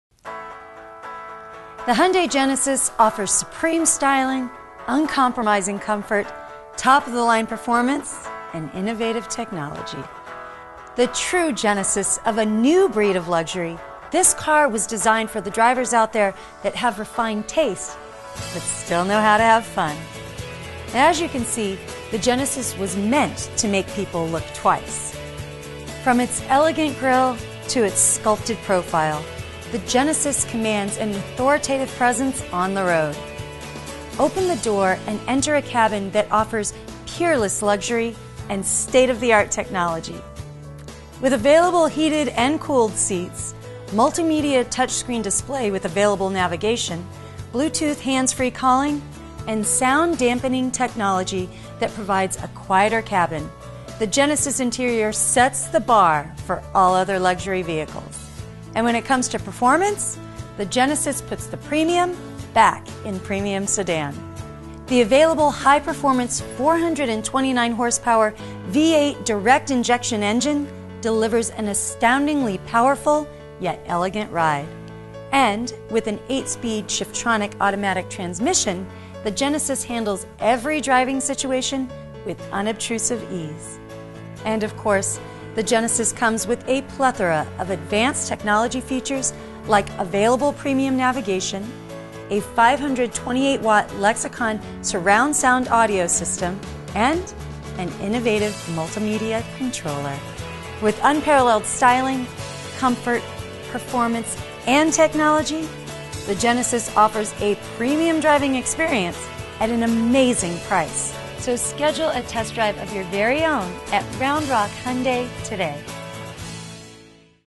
Round Rock Hyundai - 2013 Hyundai Genesis Overview
The Hyundai Genesis offers supreme styling, uncompromising comfort, top-of-the-line performance, and innovative technology. This video talks about its incredible features.